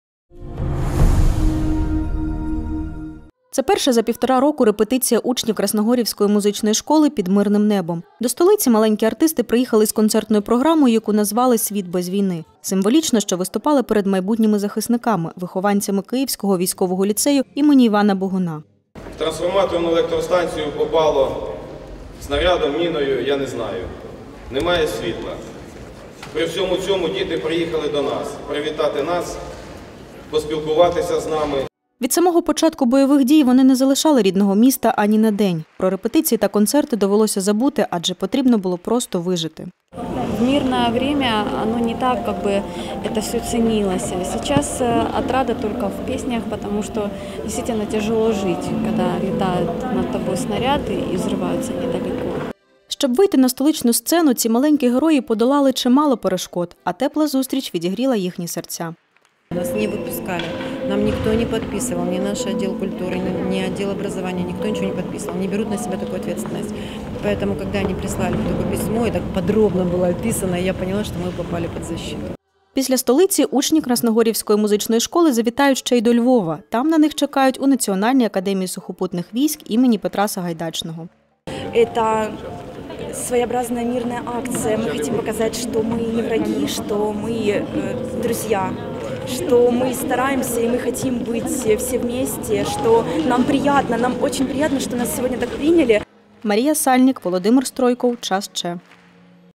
Діти Красногорівки у боротьбі за мир
Це перша за півтора року репетиція учнів Красногорівської музичної школи під мирним небом. До столиці маленькі артисти приїхали із концертною програмою, яку назвали «Світ без війни». Символічно, що виступали перед майбутніми захисниками – вихованцями Київського військового ліцею ім. Івана Богуна.